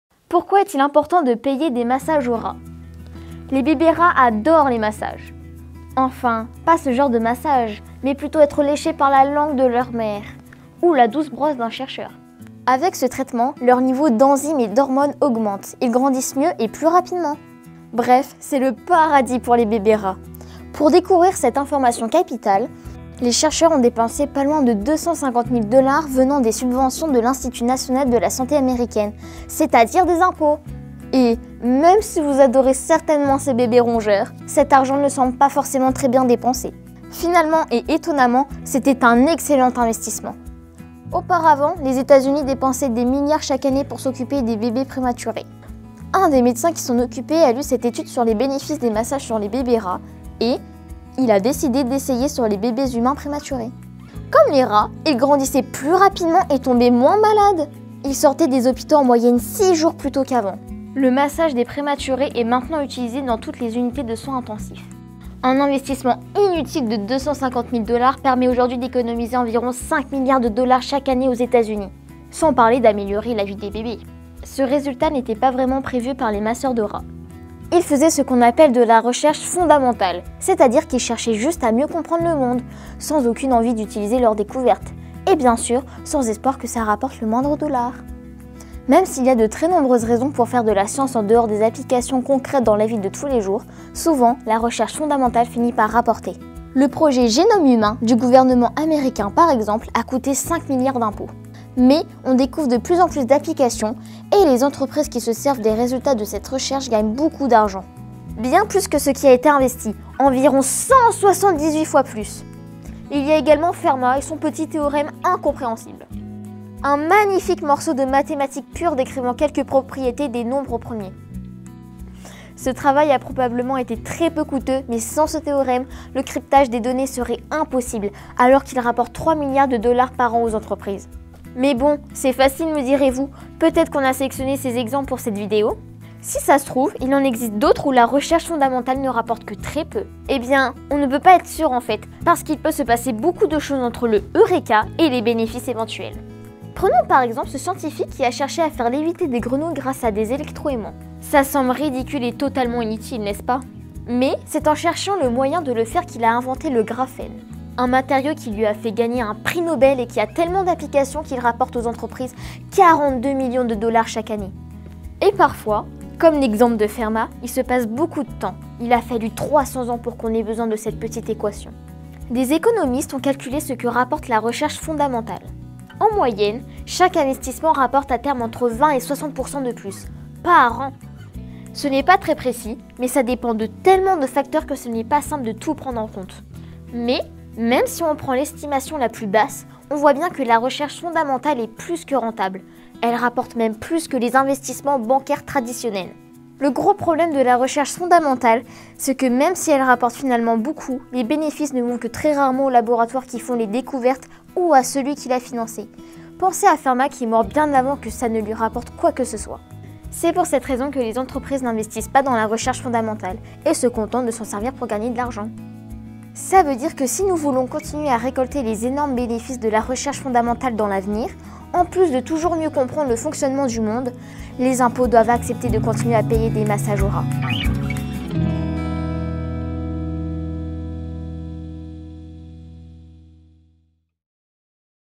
🔬 - Pourquoi faut-il payer des massages aux rats ?
La recherche fondamentale semble souvent une perte de temps et d'argent, et pourtant c'est en fait un très bon investissement.